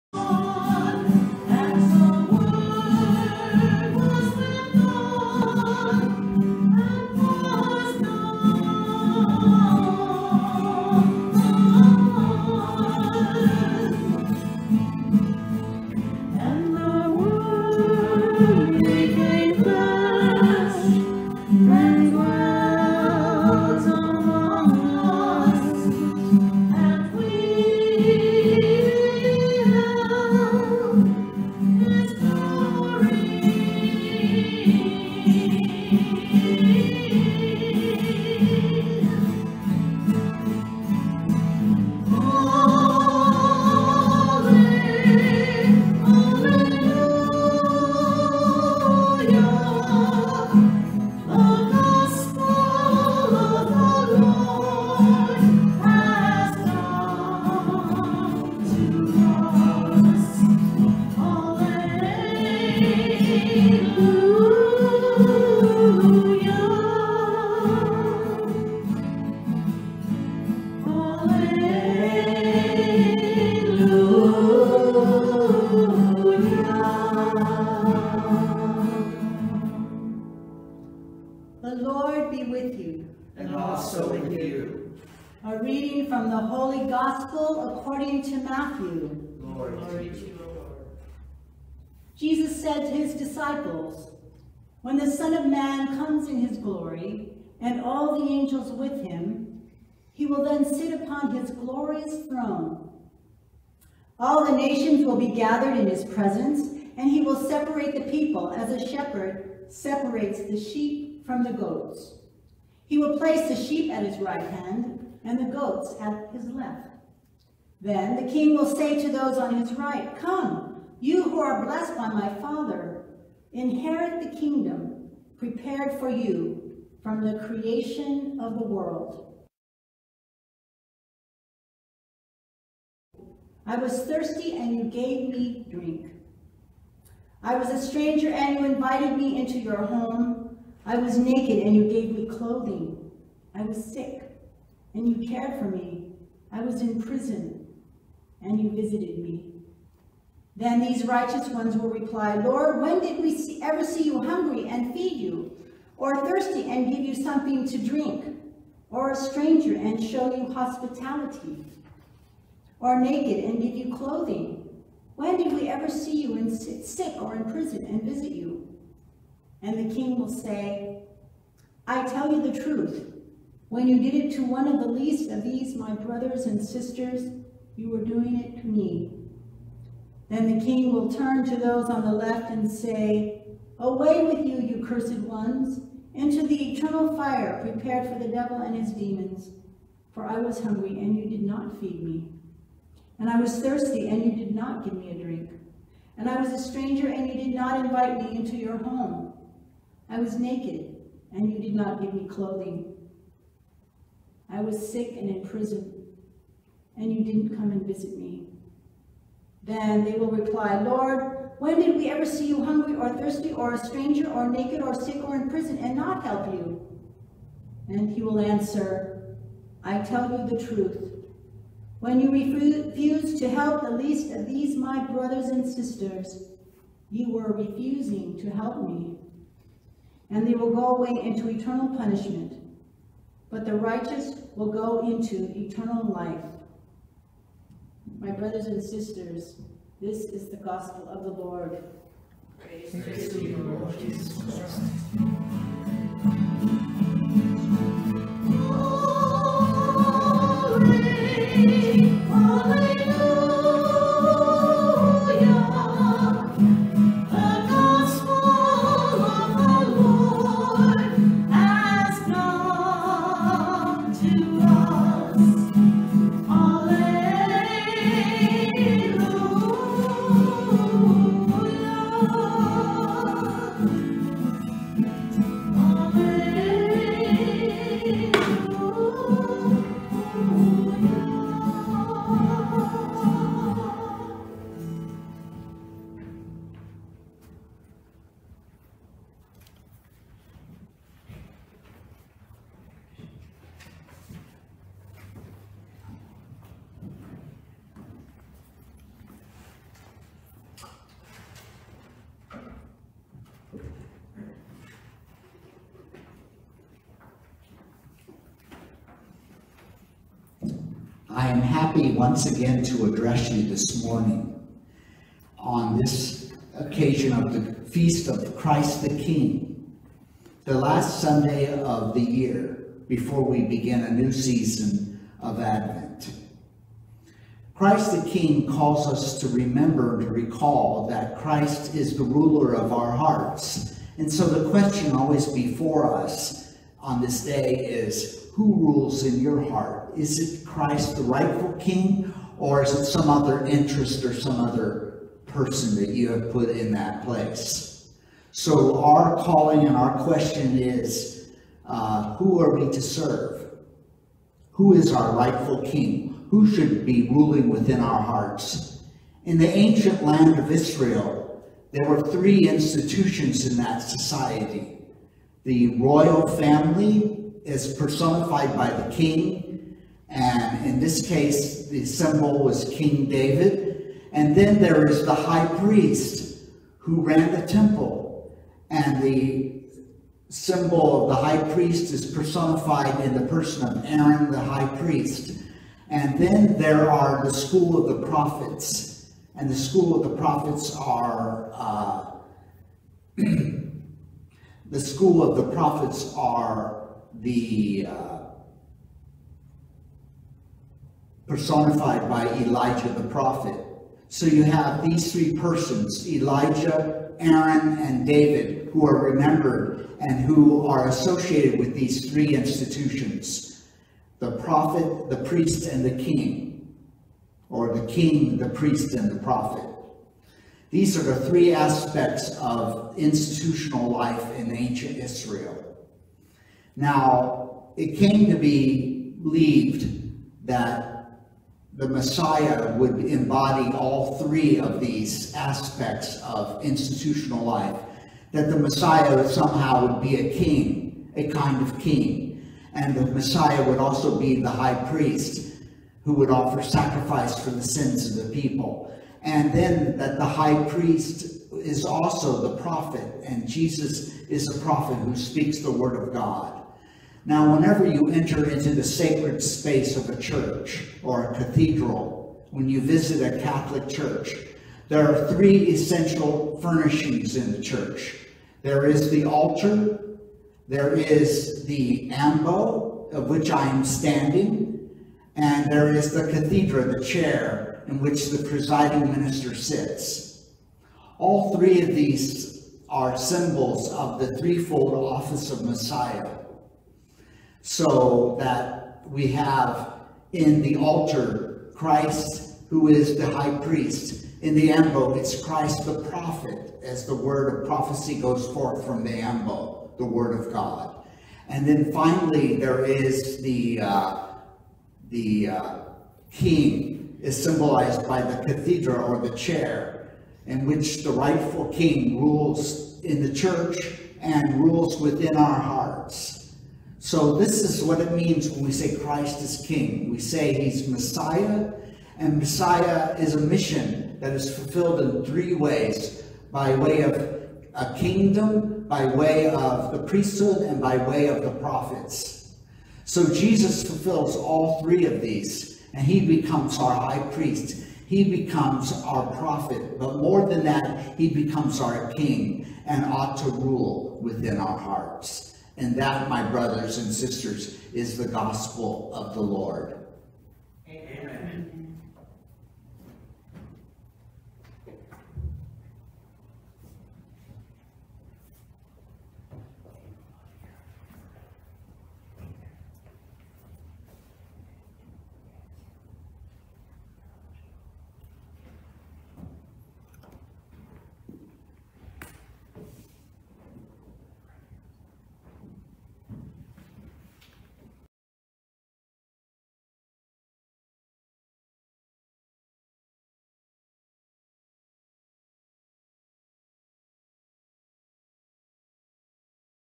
"Christ the King" Alleluia & Homily by Bishop Peter Hickman
Nov 22, 2020 at Saint Matthew Ecumenical Catholic Church in Orange, CA.